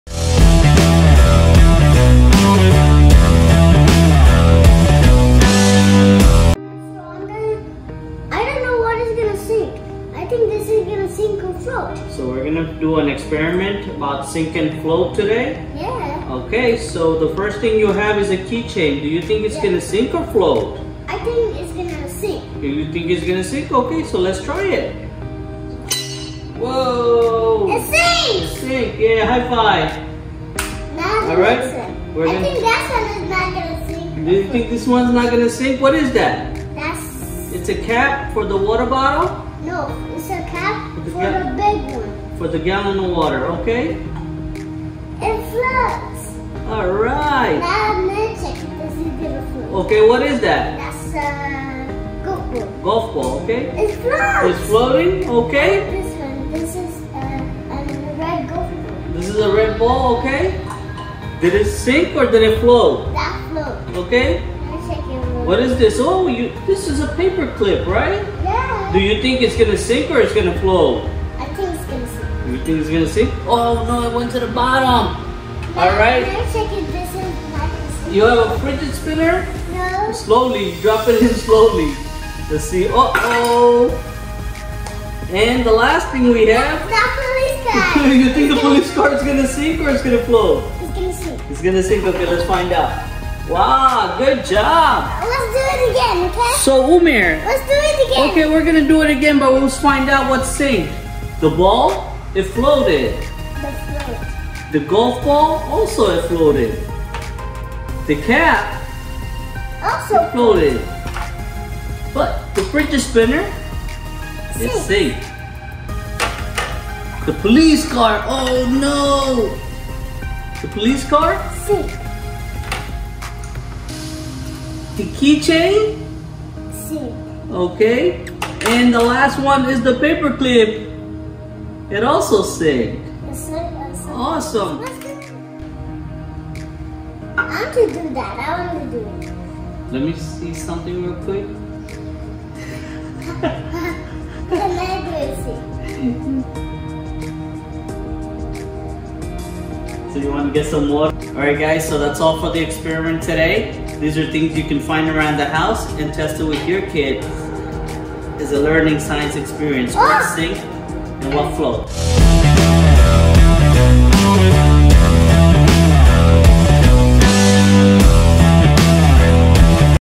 SINK OR FLOAT COOL SCIENCE EXPERIMENT FOR KIDS || DIY SCIENCE EXPERIMENT
Cool Science Experiment For Kids!
Sink or Float Experiment For Kids With Water And Houshold Iteams!!!

learn why some objects sink and others float!!!fun and easy homemade science experiments kids can do at home with everyday household objects